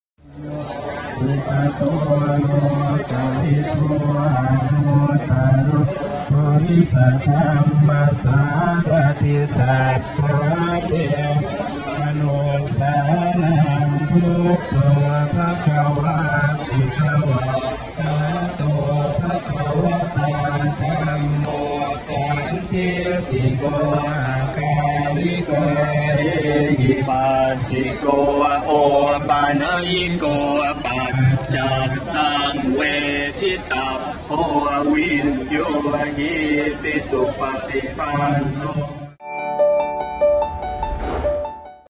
Buddha Purnima 2018: Monks offering prayers at famous Maya Devi temple in Nepal | Mijaaj News
Buddha Purnima 2018  Monks offering prayers at famous Maya Devi temple in Nepal.